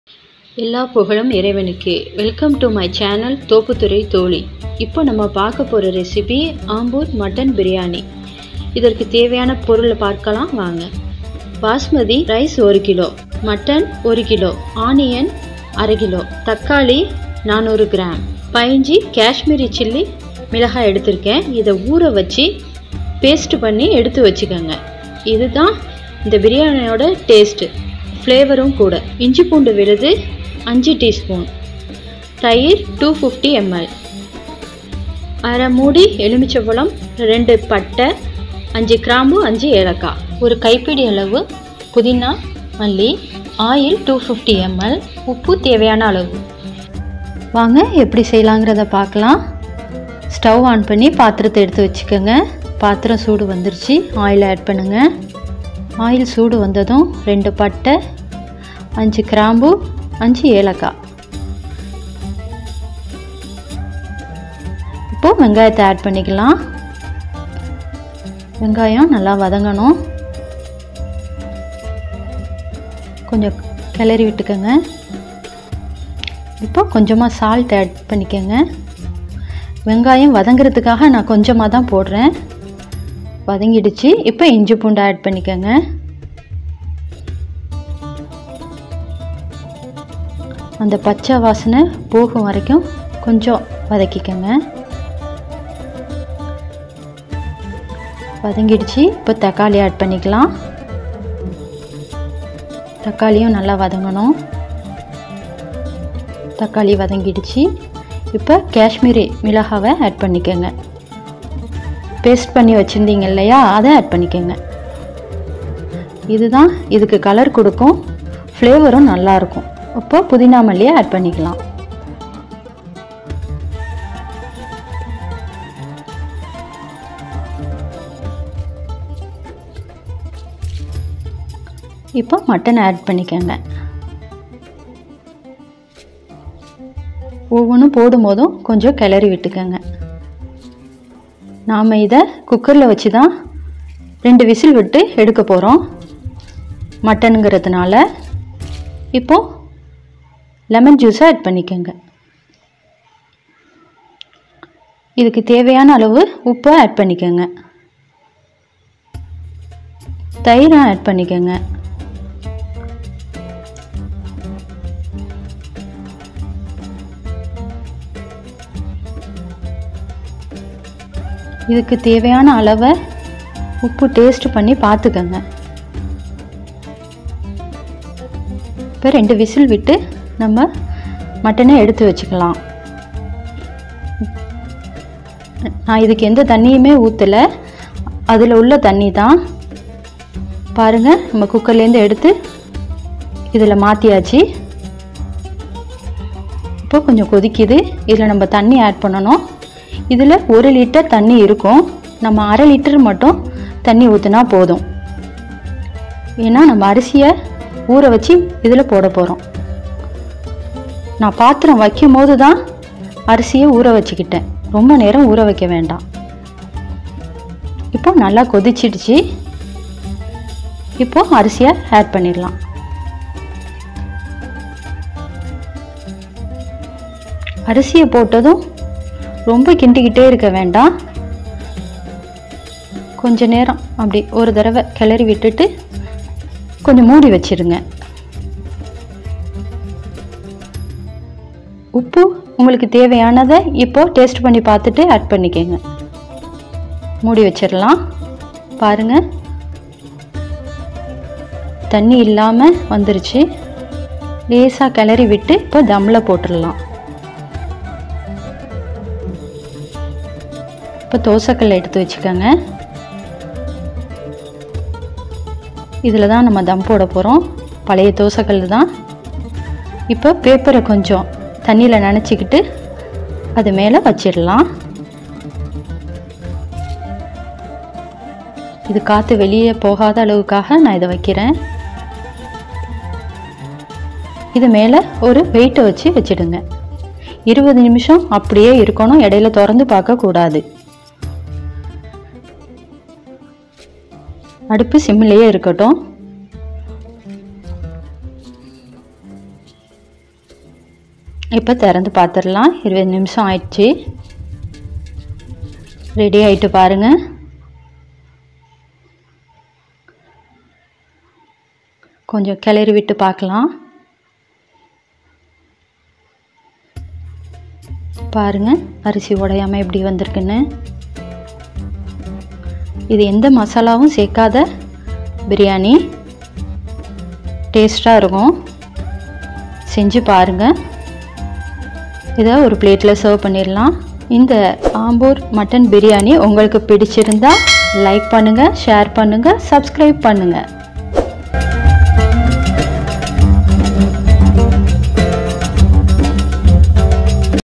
Ambur Mutton biryani Recipe in Tamil || ஆம்பூர் மட்டன் பிர்யாணி செய்வது எப்படி?
Authentic Tamilnadu Arcot Muslim Style  Mutton Biryani. legend has it that the mutton biryani was introduced to the Arcot royals through their connection with the Hyderabadi Nizams.

Ingredients:
Basmathi Rice                     1 kg
Tomato                                 400 g
Kashmiri chilly                     15
Lemon                                  1/2 pc
Cinnamon                            2
Clove                                     5
Elaichi                                   5
Coriander, Mint leaves       1/4